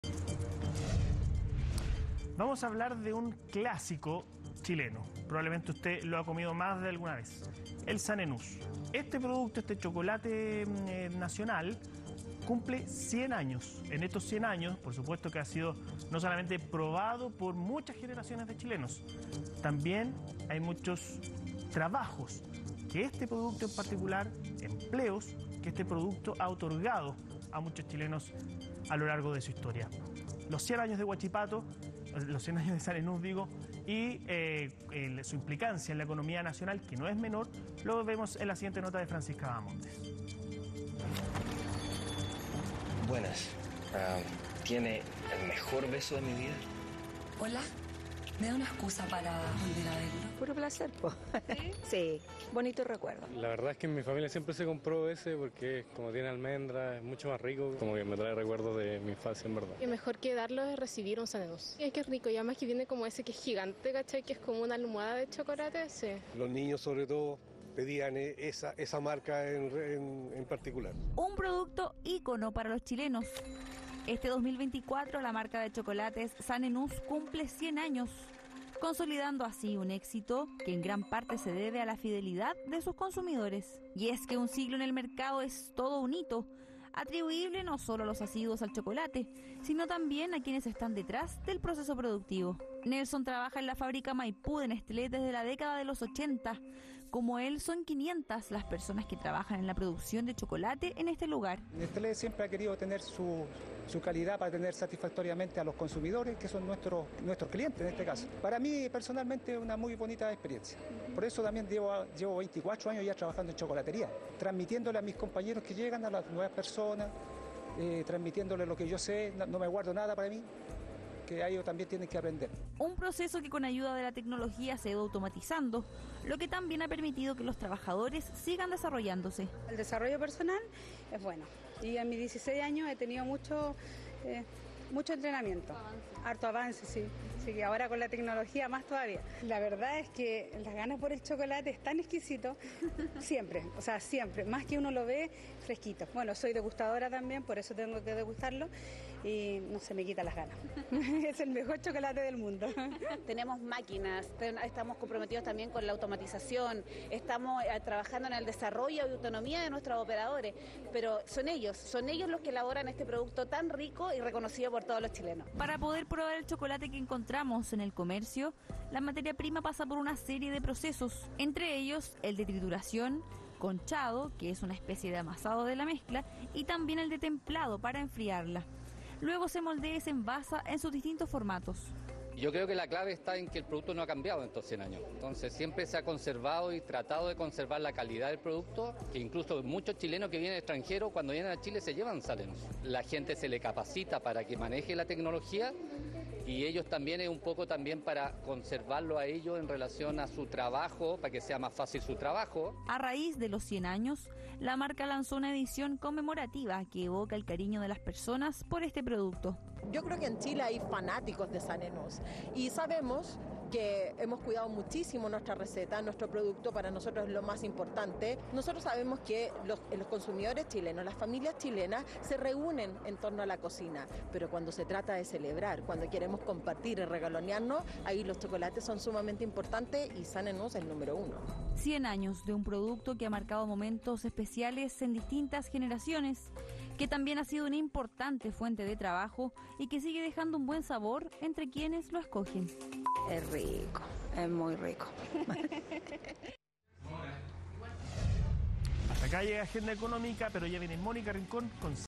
SAHNE-NUSS® CUMPLE 100 AÑOS
Descripción:  En el marco de la celebración de los 100 años de nuestra marca Sahne-Nuss, CNN Chile realizó este reportaje que refleja como este icónico chocolate ha sido parte de la historia de varias generaciones de consumidores y también como ha aportado a la generación de empleo e innovación.